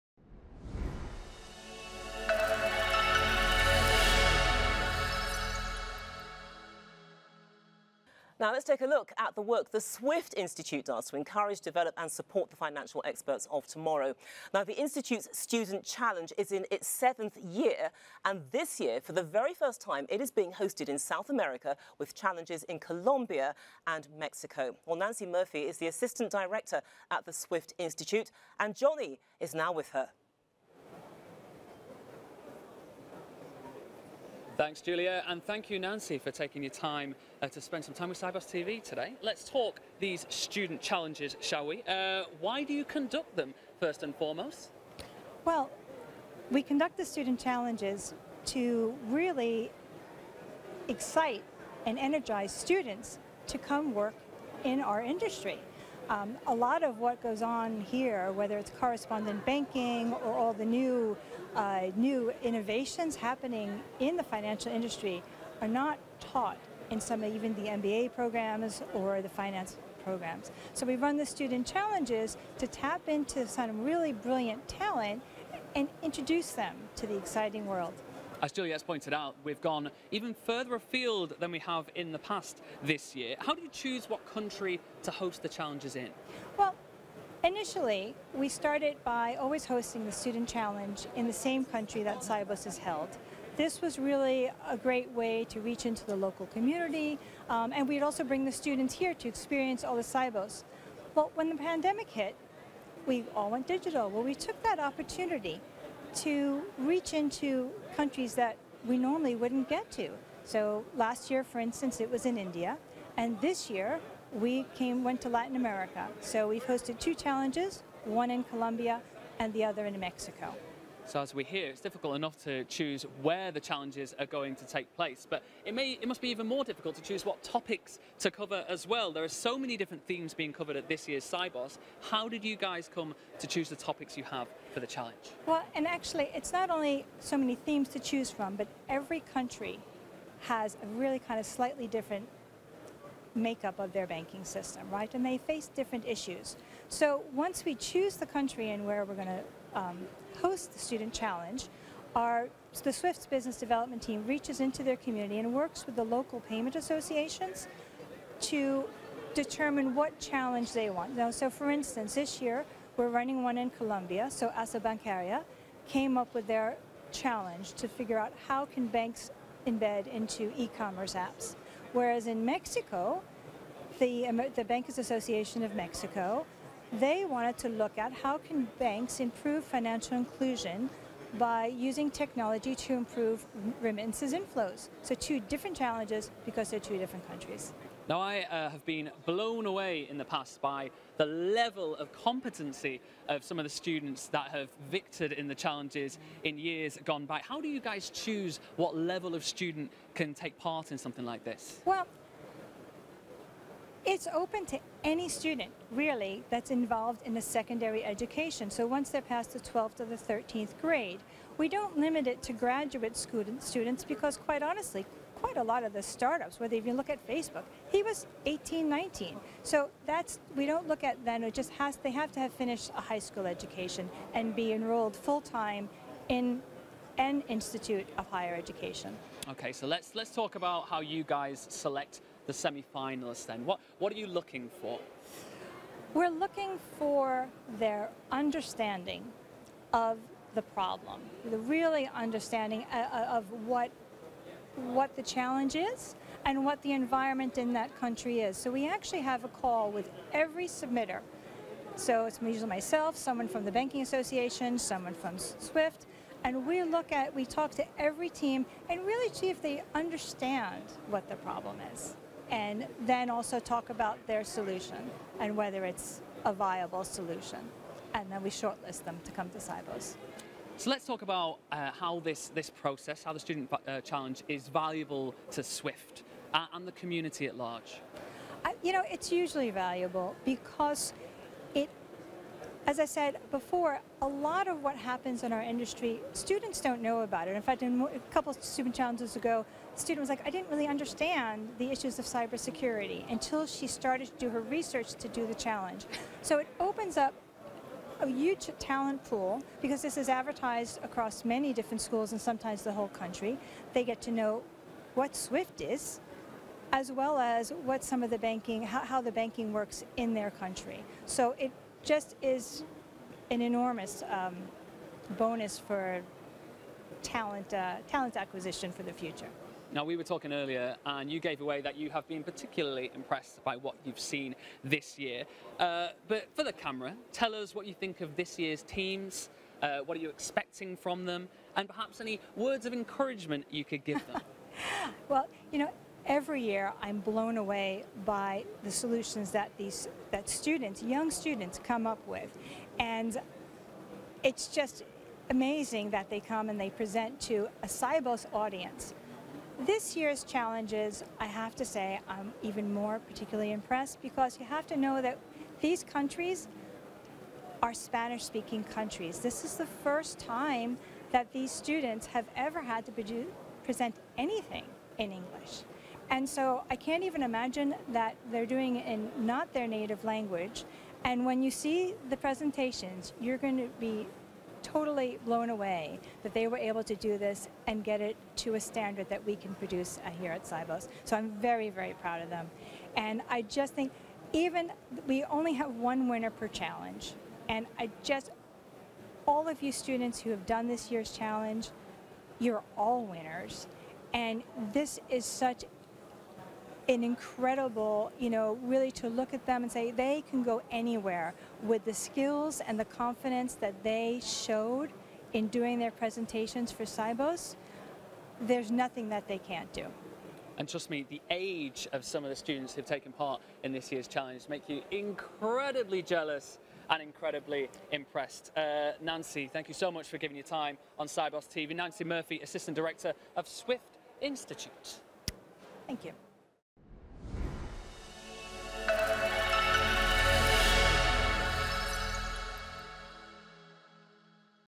Sibos TV: How Swift supports the talent of tomorrow - Oct 2022
The Swift Institute Student Challenge is now in its seventh year and is being hosted in South America for the first time in 2022, with challenges in Colombia and Mexico. Nancy Murphy, Assistant Director at the Swift Institute, joins Sibos TV to explain the value this year's students will derive from the challenges as the Institute helps support the financial experts of tomorrow.